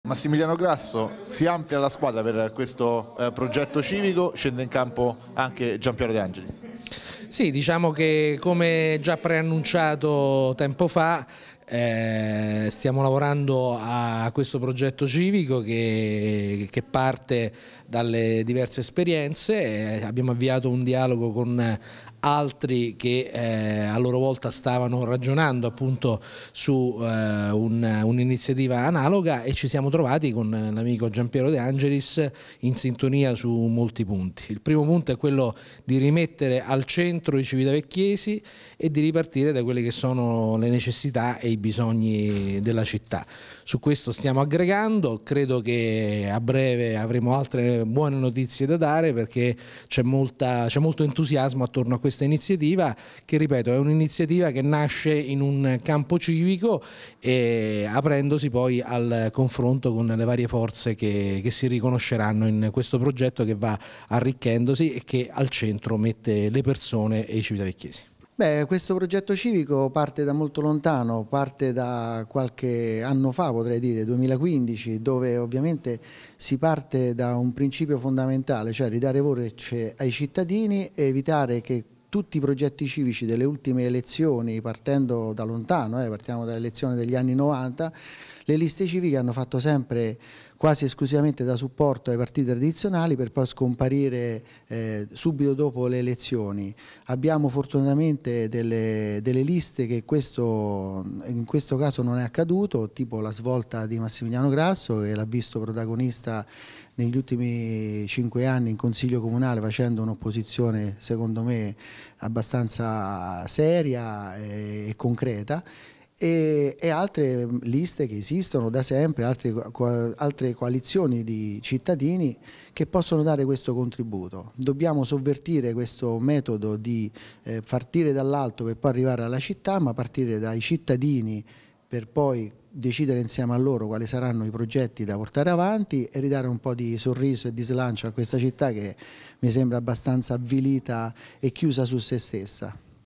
De Angelis: abbiamo ripreso un progetto che parte da lontano
Alla squadra del leader de La Svolta Massimiliano Grasso il punto di riferimento, si è aggiunto l’ex assessore al Bilancio della giunta Moscherini De Angelis.